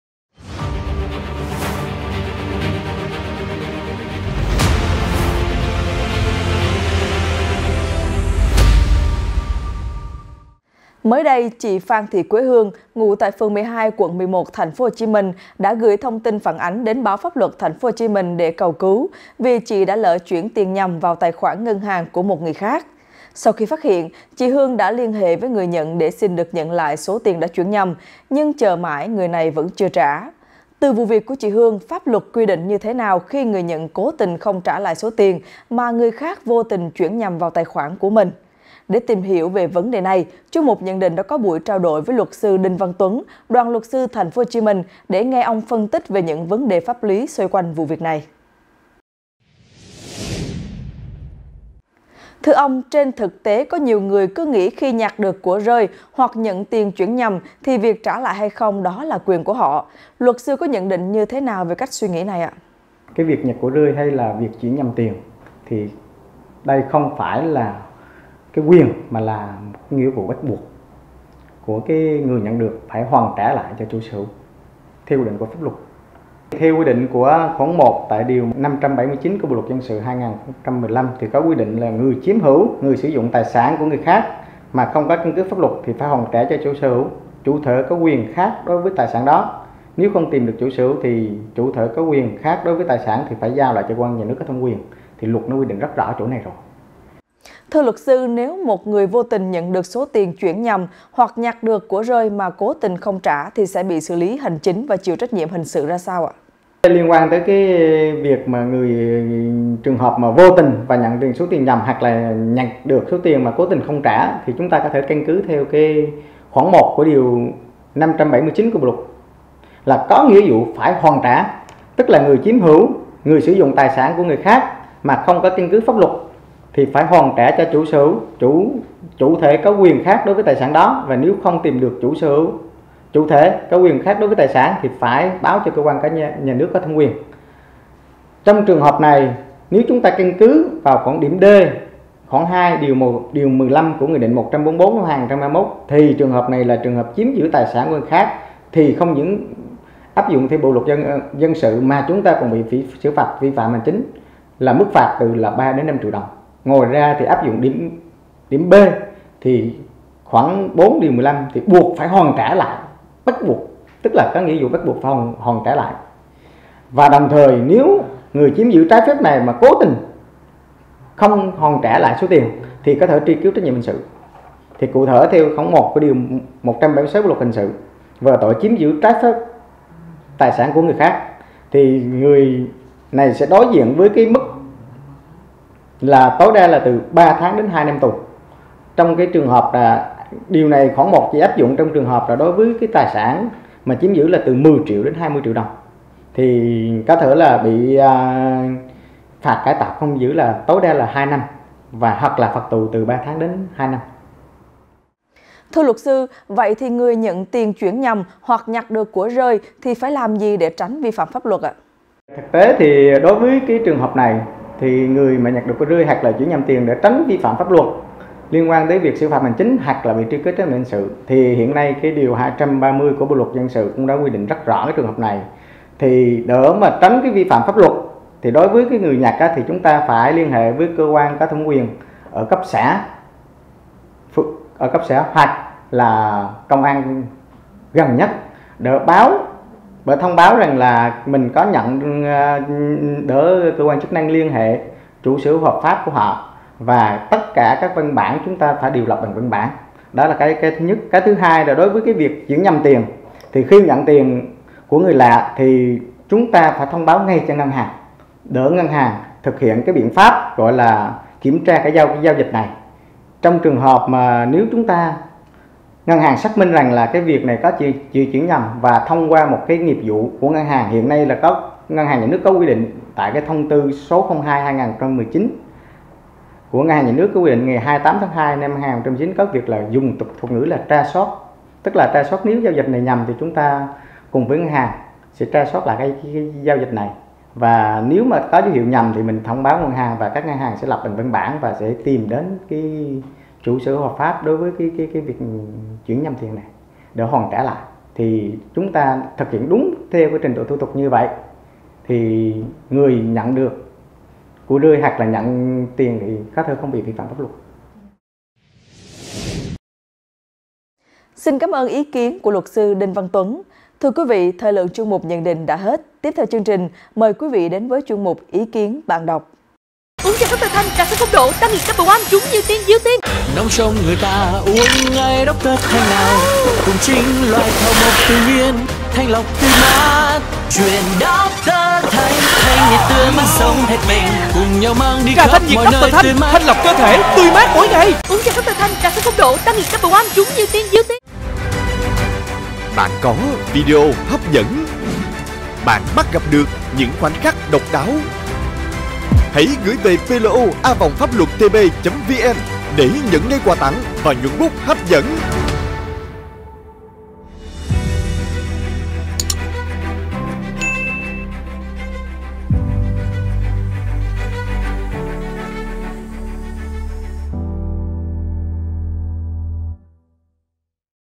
Nhận tiền chuyển nhầm vào tài khoản không trả, bị xử lý sao? - Góc nhìn chuyên gia - PLO
Pháp luật quy định như thế nào khi người nhận cố tình không trả lại số tiền mà người khác vô tình chuyển nhầm vào tài khoản của mình. Để tìm hiểu về vấn đề này, chuyên mục Nhận Định đã có cuộc trao đổi với Luật sư Đinh Văn Tuấn, Đoàn Luật sư TP.HCM về những vấn đề pháp lý xoay vụ việc trên.